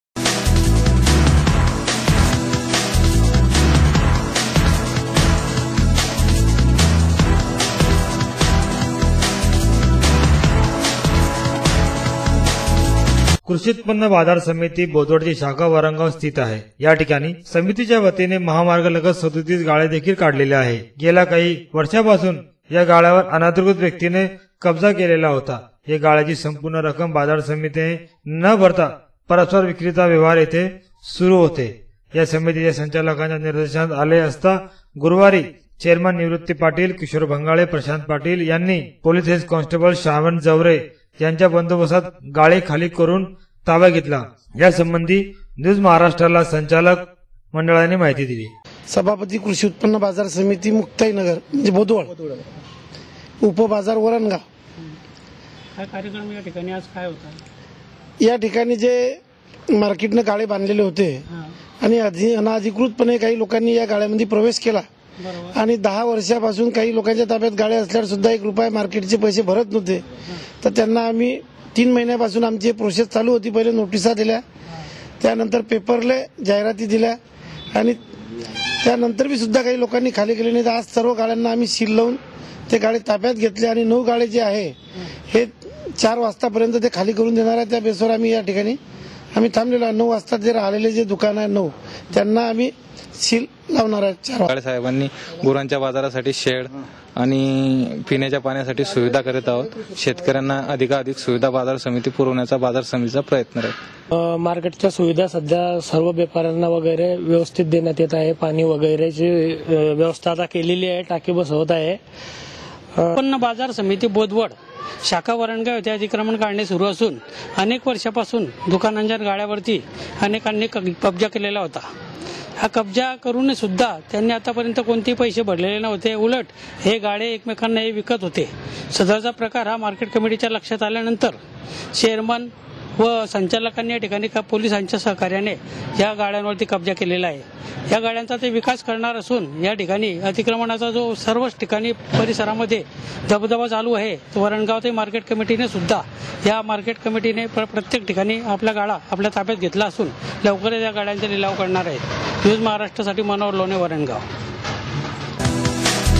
VARANGAON
बाजार समितीने केले अतिक्रमण गाळे जप्त.